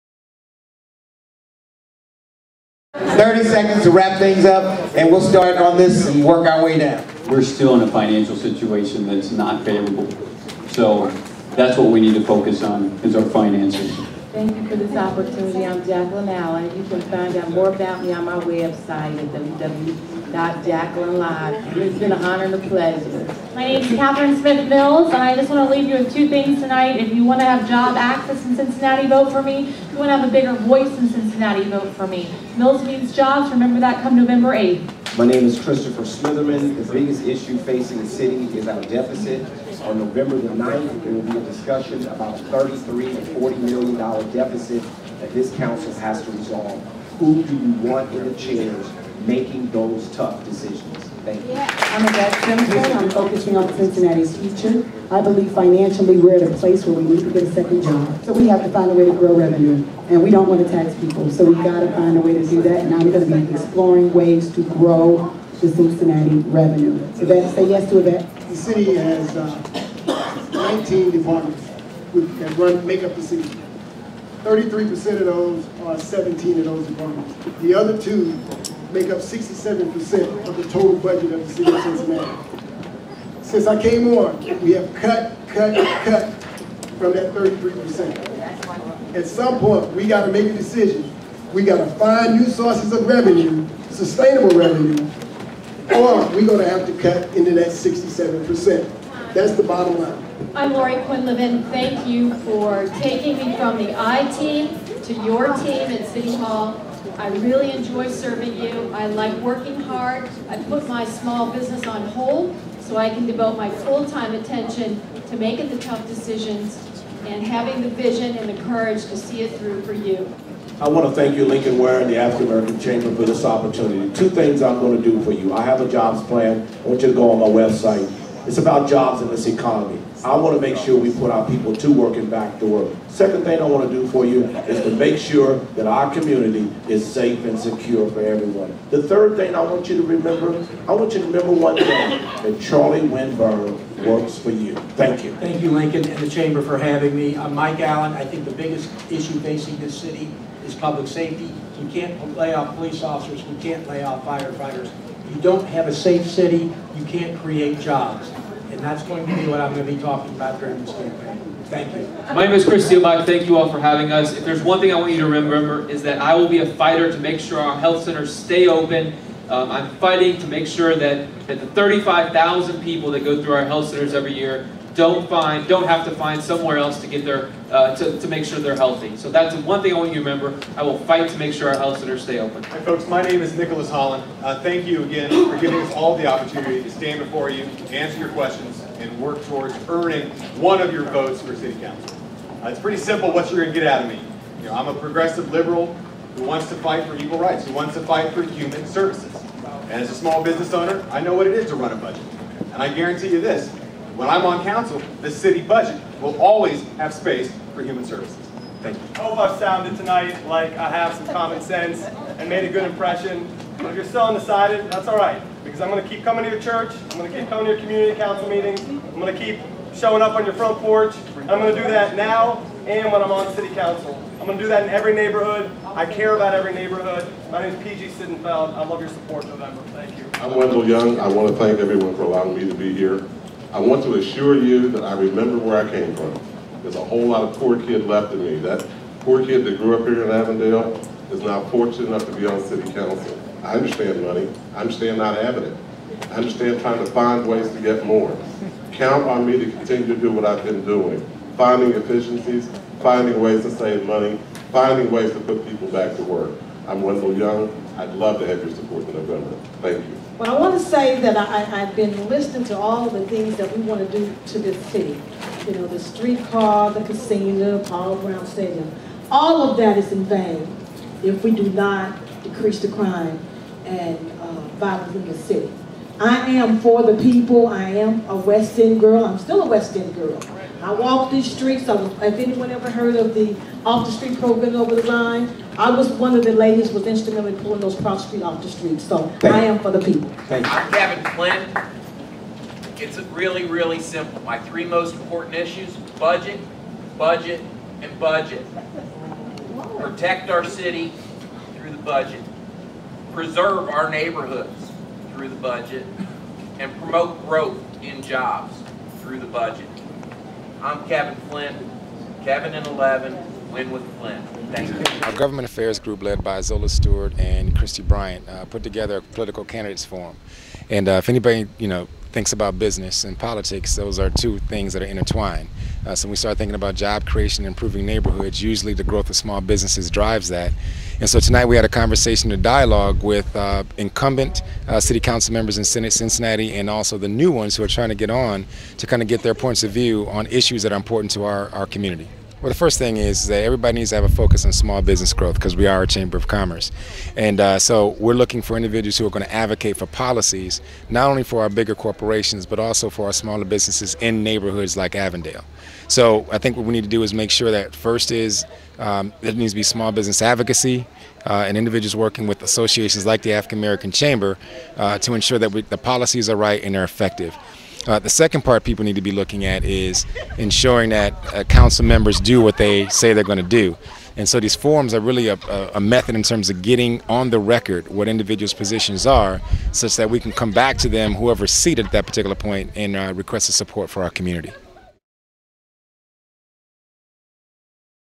Cincinnati City Council Candidate's Forum 2011
I attended The Cincinnati City Council Candidate's Forum on September 14, 2011. It was very informative! This event held at the Avondale Pride Center and was hosted by the African American Chamber of Commerce of Greater Cincinnati/Northern Kentucky Government Affairs Committee and WBDZ 1230 AM Radio Station

T.V. and Radio Talk Show Host Lincoln Ware was the moderator for this forum which also presented Candidates for Judgeship in Cincinnati and Hamilton county, Ohio

After hearing from the Candidates African American Chamber of Commerce President & CEO Sean Rugless spoke to me about the event.

List of Candidates that attended this Candidate's Forum:

Incumbents:
Councilman Cecil Thomas

Candidates:
Jason Riveiro

Jacqueline Allen

P.G. Sittenfeld

The Greater Cincinnati and Northern Kentucky African American Chamber of Commerce